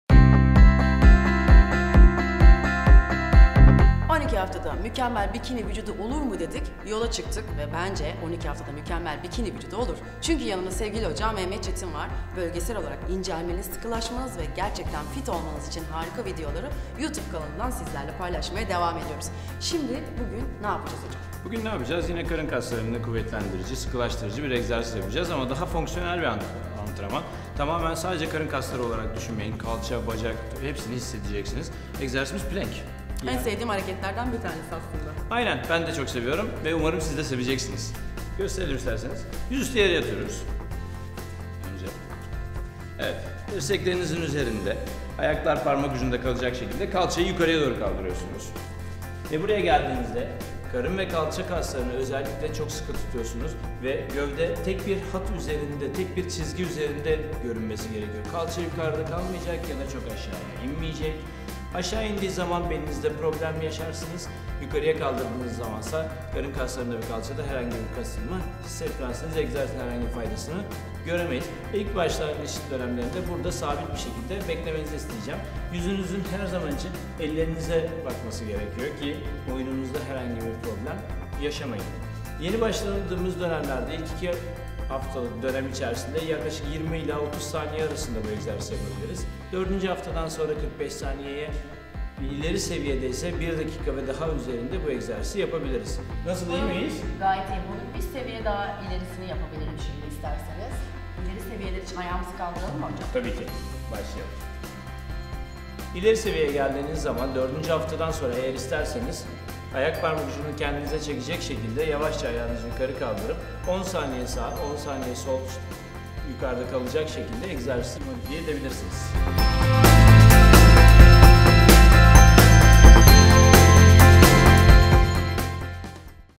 Plank | 8 Haftada Değişim Spor Serisi- Mehmet Çetin
Plank: 3 Set x 10 - 15 Saniye
Her set arası 30 saniye mola.